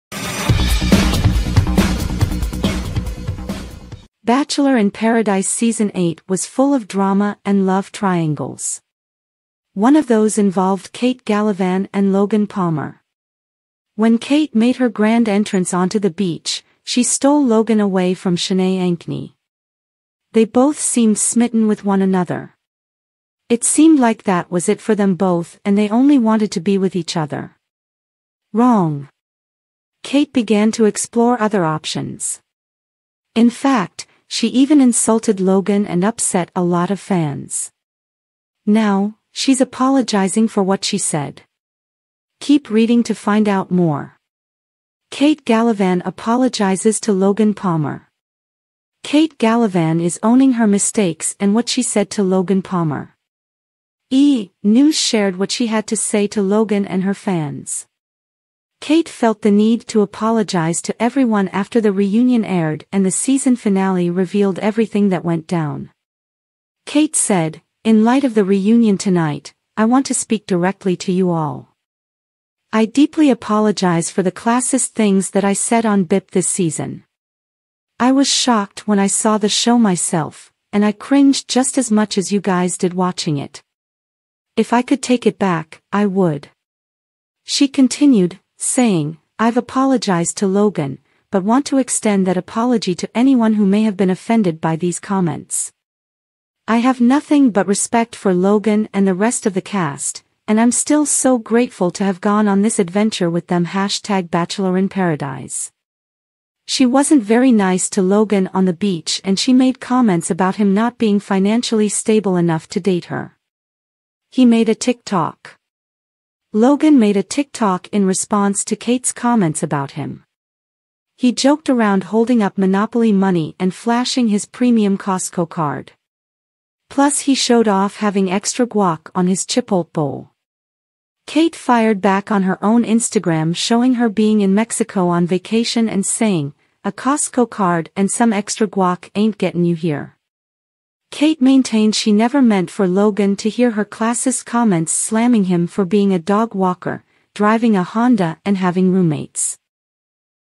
Kate Gallivan Issues Apology To Logan Palmer After Disrespect
The eighth season of The Bachelor in Paradise was full of love triangles and drama.Kate Gallivan and Logan Palmer were a part of one of them.Kate stole Logan from Shanae Ankney as she made her grand entrance onto the beach.They appeared to be in love with one another.They seemed to have reached their limit, and all they wanted was to be together.Wrong.Kate started looking into other options.She even made fun of Logan, upsetting a lot of fans.She is now apologizing for her words.Continue reading to learn more.